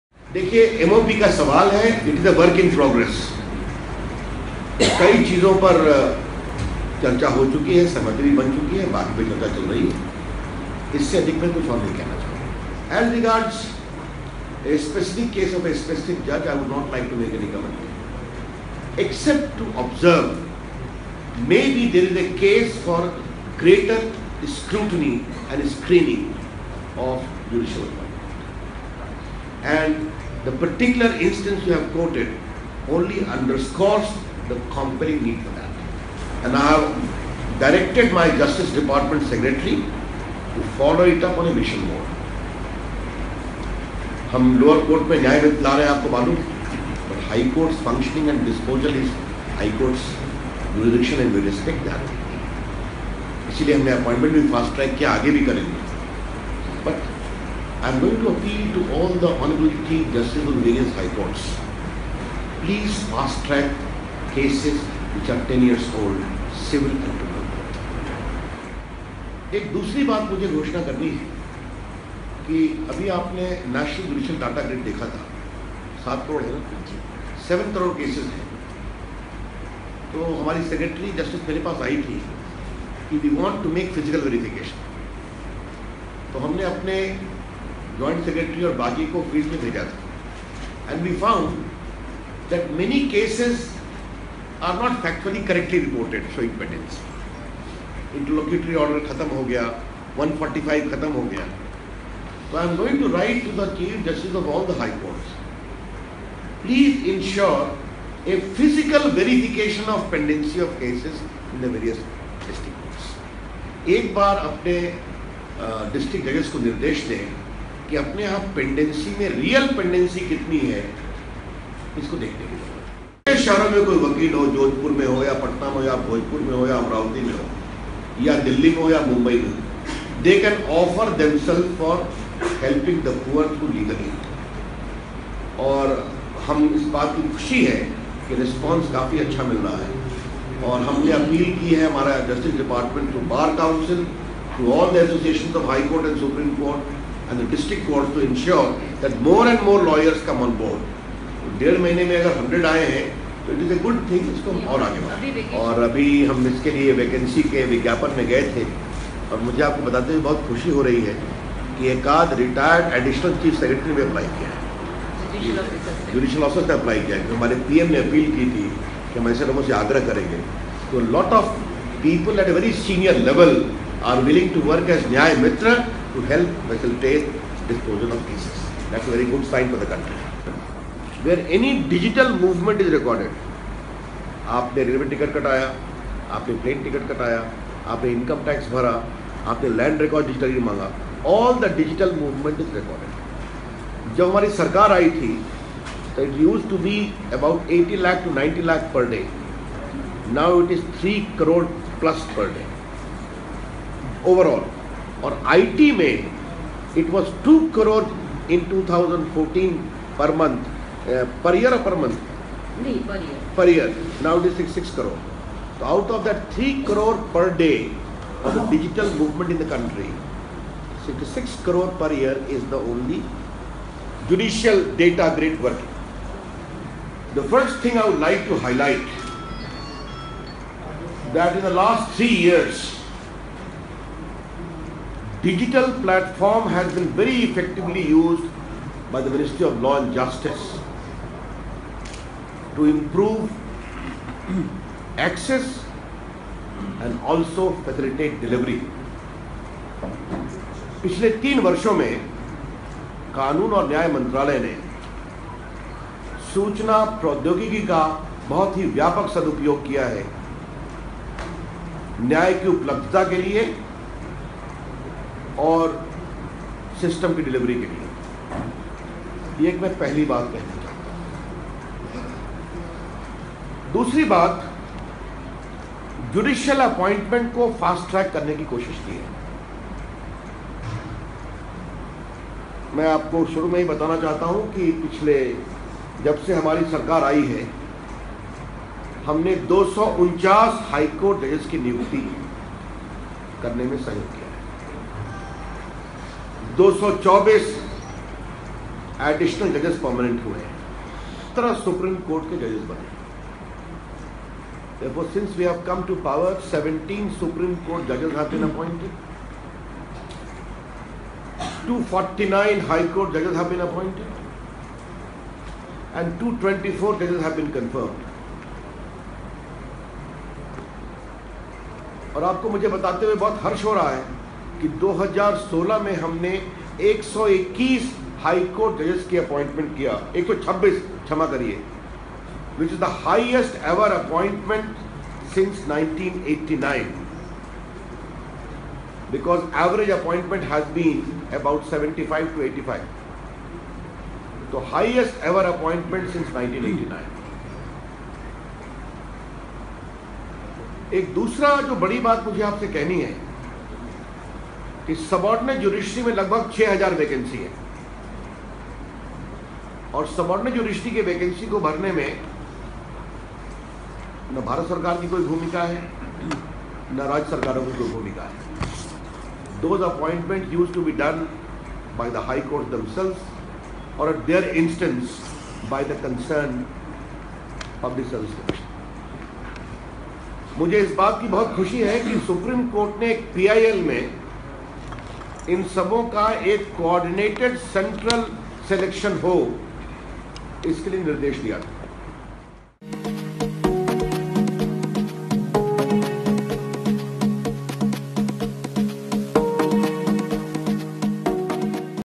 Ravi Shankar Prasad On New Initiatives By The Ministry of Law & Justice
Video: Jitender Gupta; Editing: Rupesh Malviya